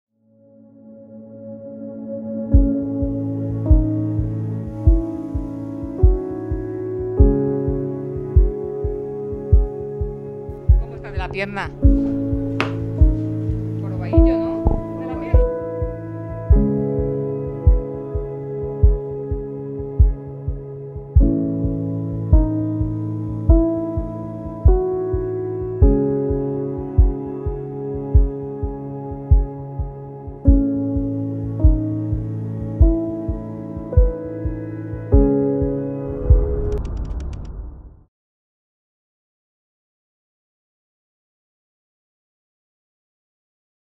Kiko Rivera desvela cómo se encuentra tras confirmar su problema de salud
El hijo de Isabel Pantoja ha revelado el nuevo varapalo que ha sufrido y que tiene que ver con su rodilla.